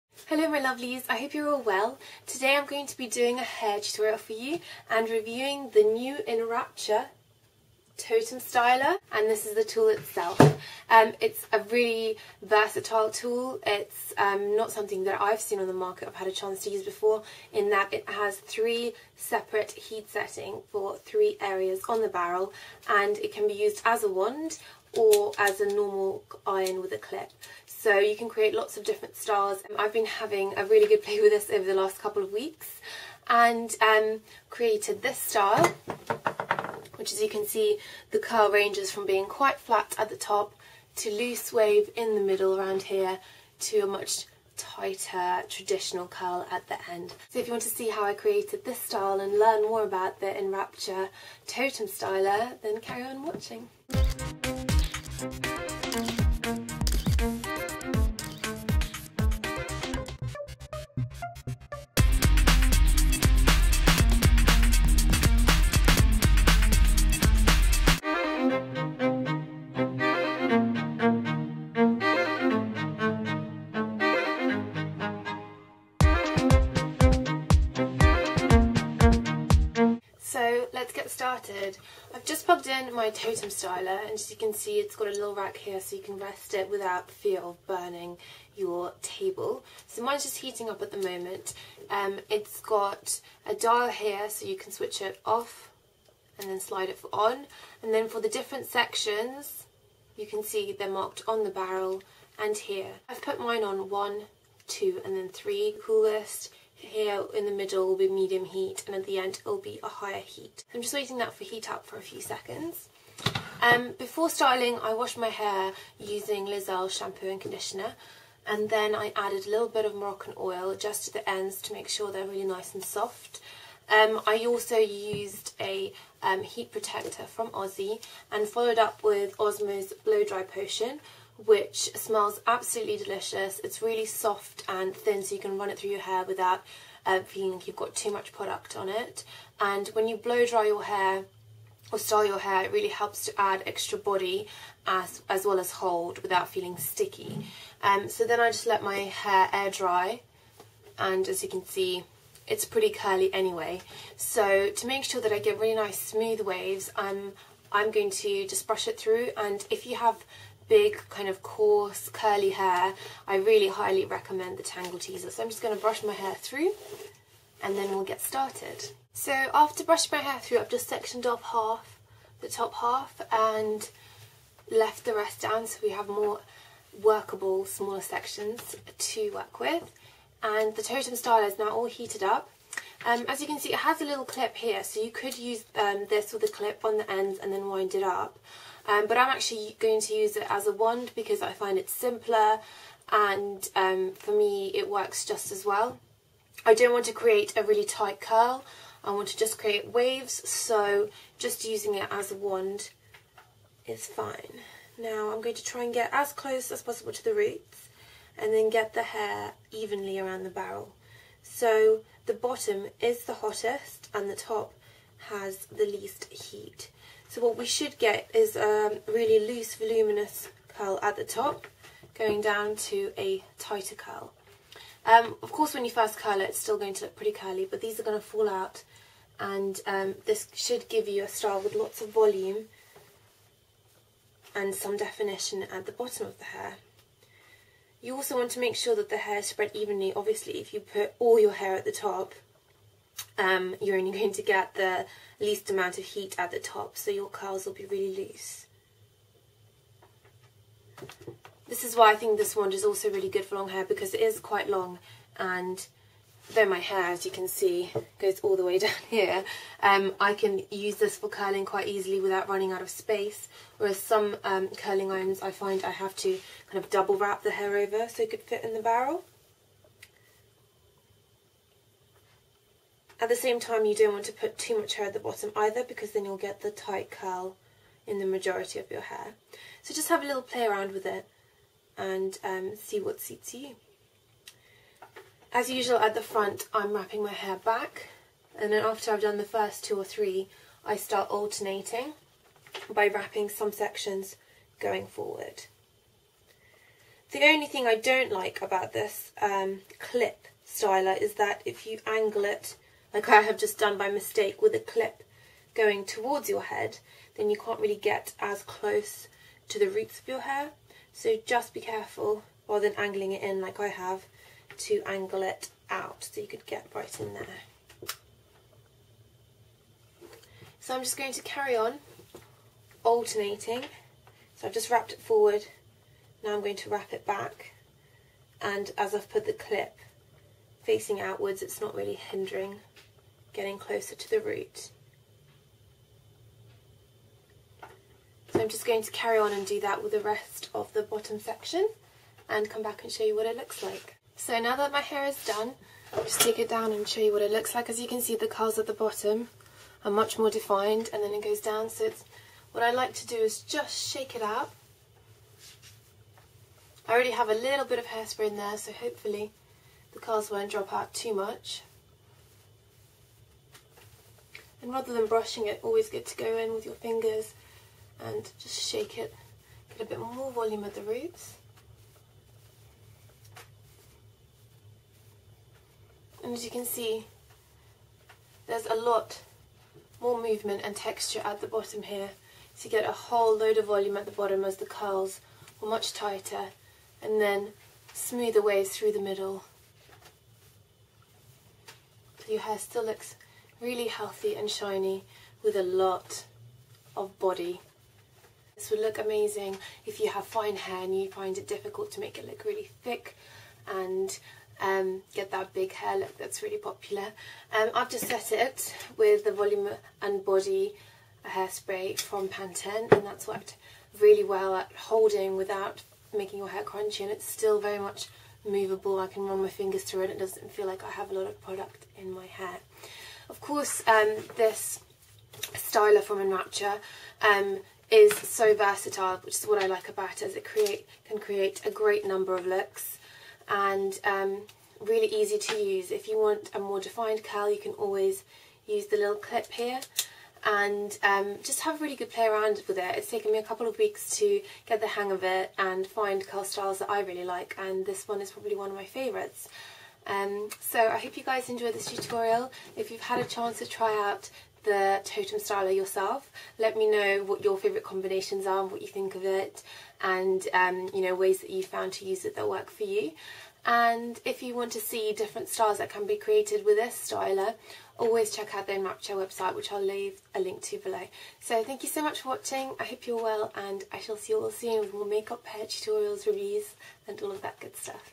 Big Bouncy Curls with Enrapture Totem Styler
The Totem Styler from Enrapture is a unique, versatile tool that lets you create a myriad of styles with it's three separate heat settings!